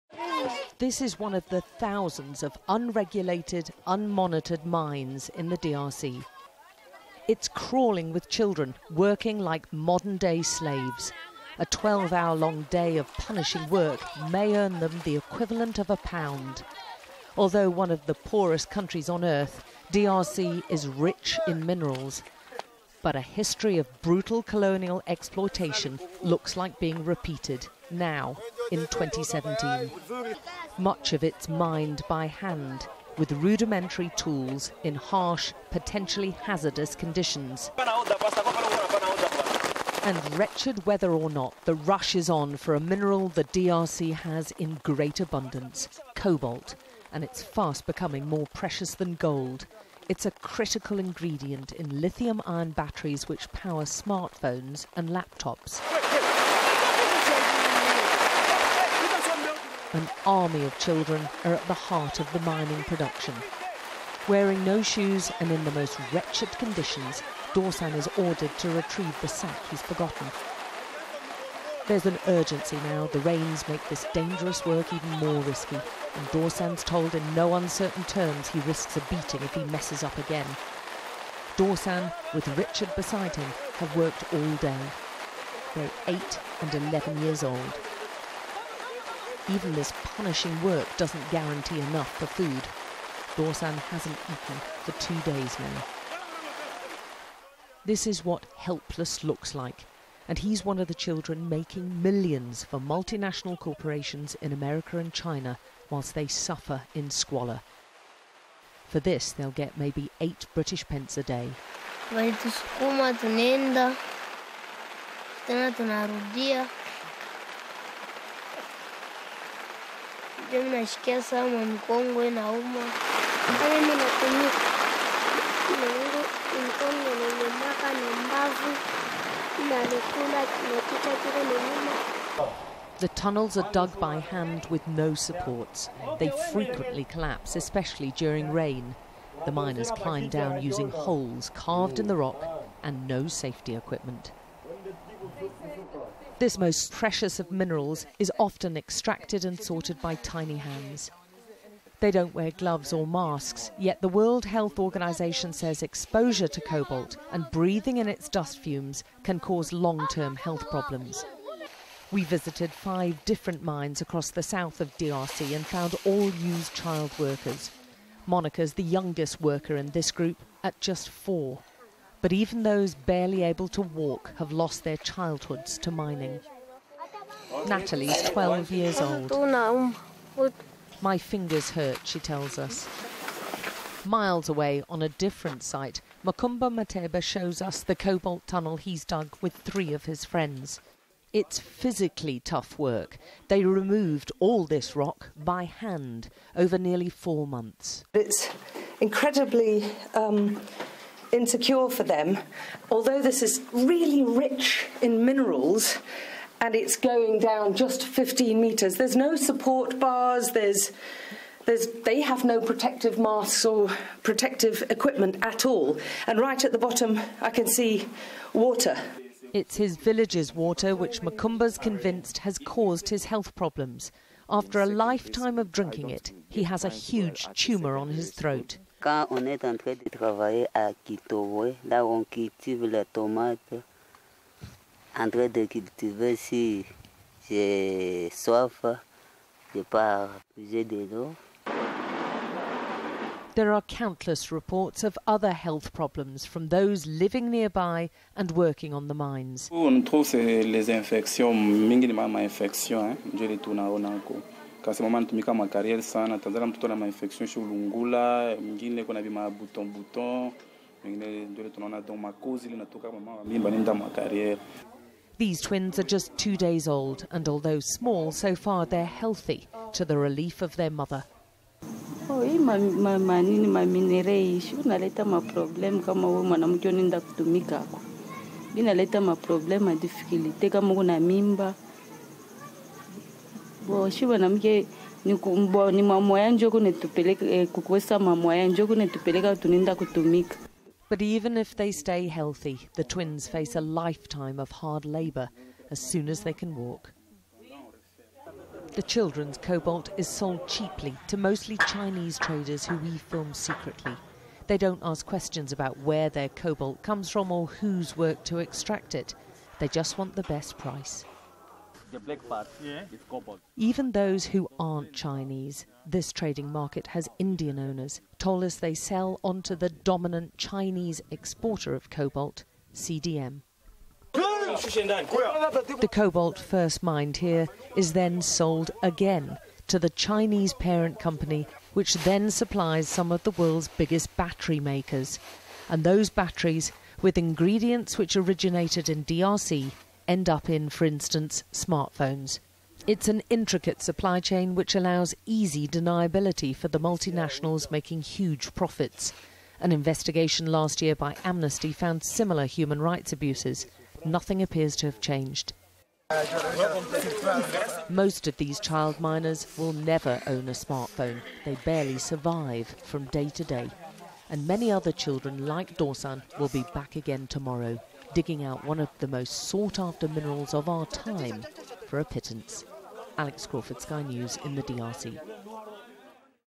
Special report: Inside the Congo cobalt mines that exploit children
It is an essential part of most mobile gadgets sold around the world and demand for cobalt is soaring. But the process of extracting the mineral from the earth comes at a huge cost.
Credit: Sky News